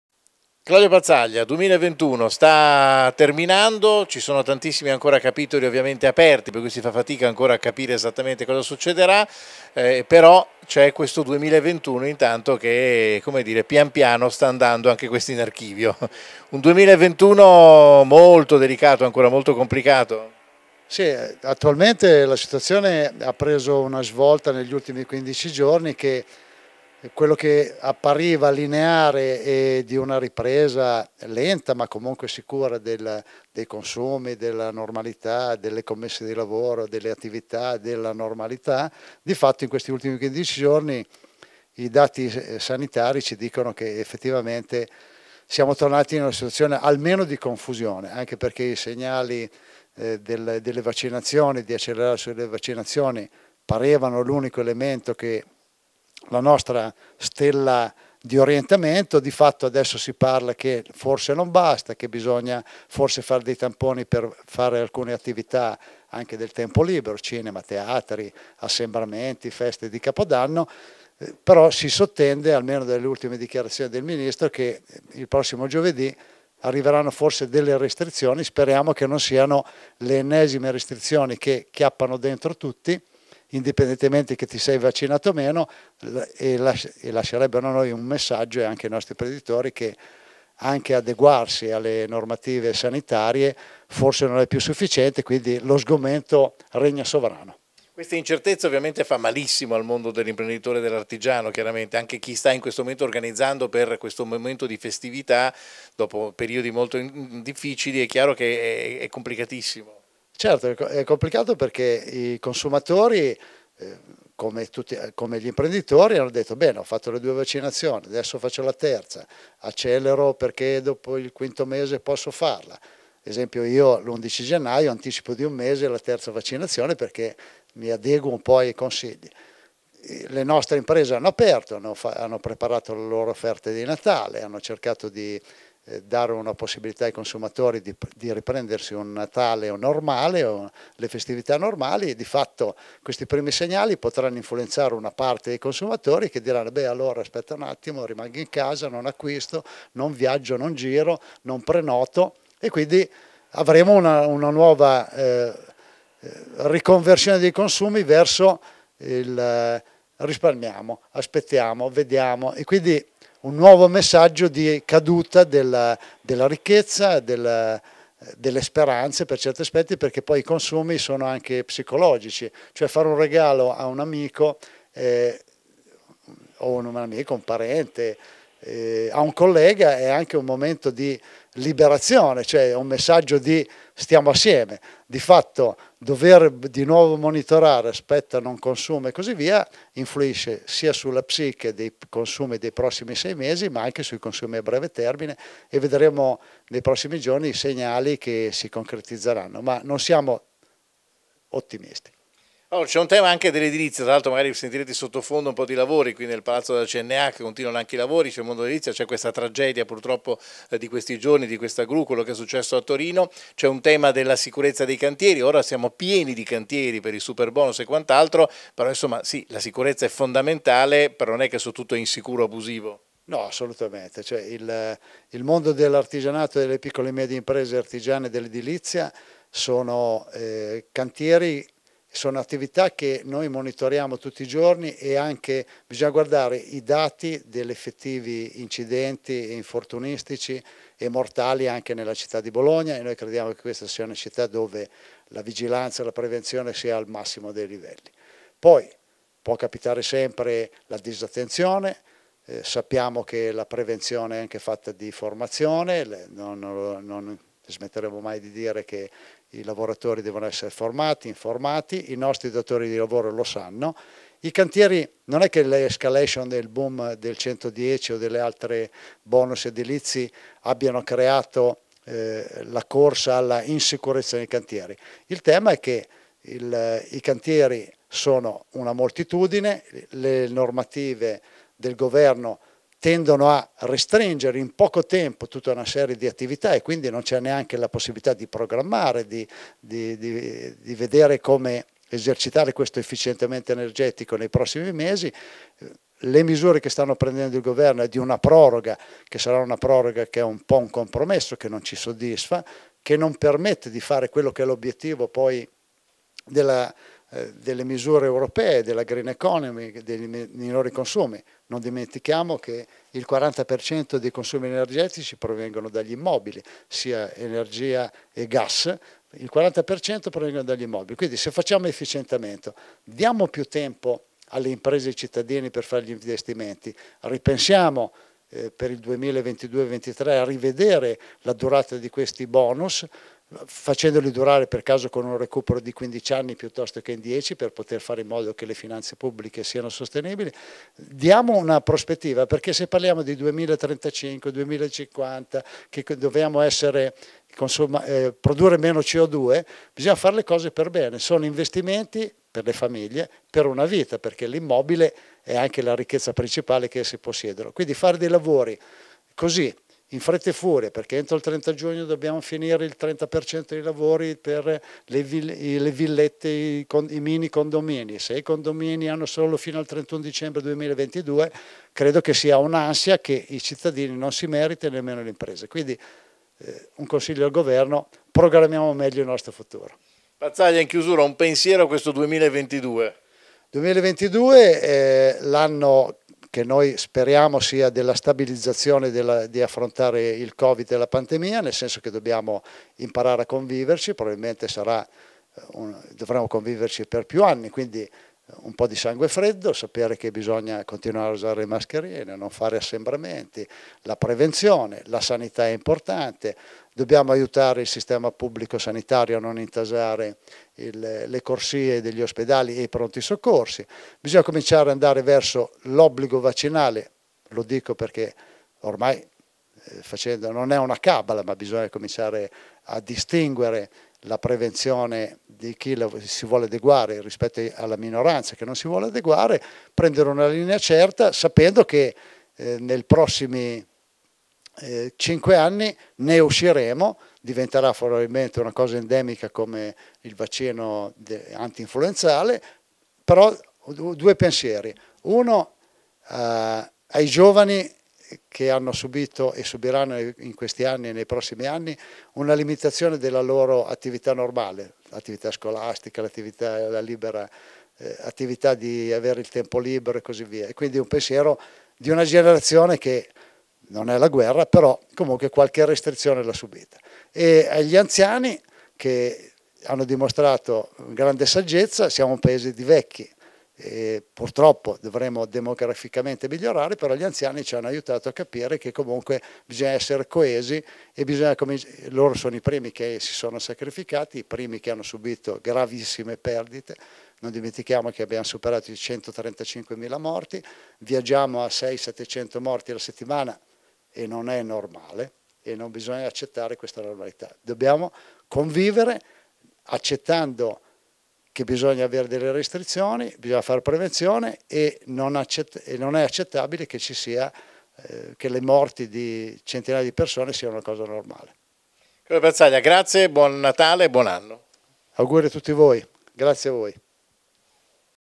Intervista a Claudio Pazzaglia Direttore CNA Bologna: analisi del 2021
Nell'intervista a Claudio Pazzaglia, Direttore CNA di Bologna, un resoconto   del 2021, un commento sulla situazione attuale e del prossimo futuro, che vede ancora purtroppo diverse incertezze.